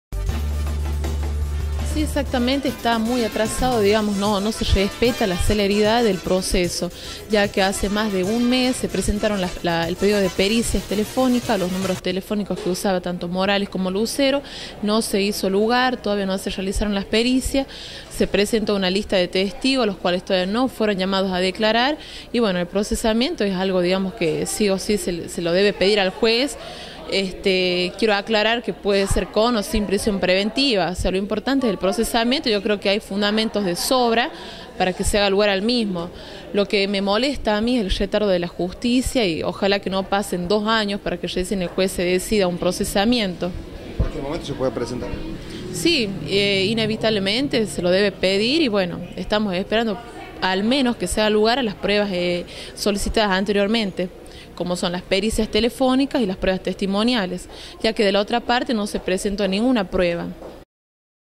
NOTIRIOJATV: LUCERO MORALES LENTO EL PROCESO
LA ABOGADA MANRIQUE DENUNCIA LENTITUD EN EL PROCESO CASO LUCERO CONTRA MORALES